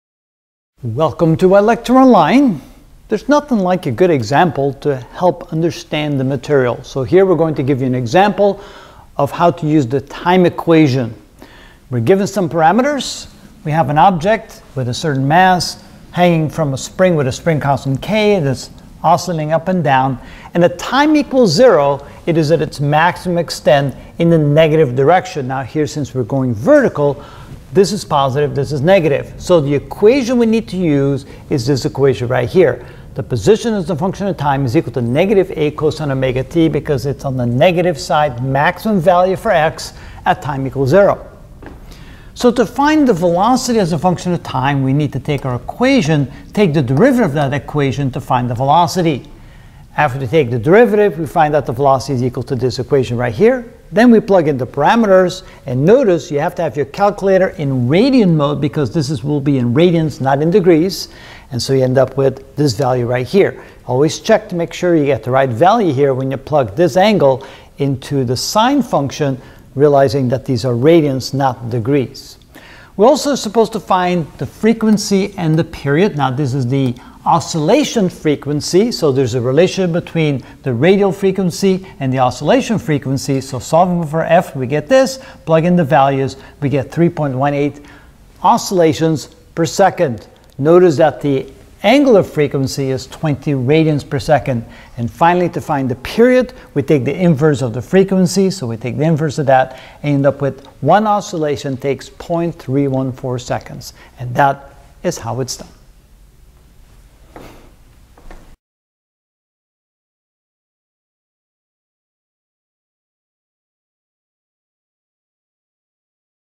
Physics Review: Simple Harmonic Motion #6 Time Equation Example
We will solve an example of using the time equation to find v(t)=?, f=?, and T=? of a mass attached to a hanging spring given t=2s, m=0.5kf, k=200N/m, and A=10cm.

Title search on other plateforms:  Physics Review: Simple Harmonic Motion #5 Initial Conditions

Title search on other plateforms:  Physics Review: Simple Harmonic Motion #7 Damping Basics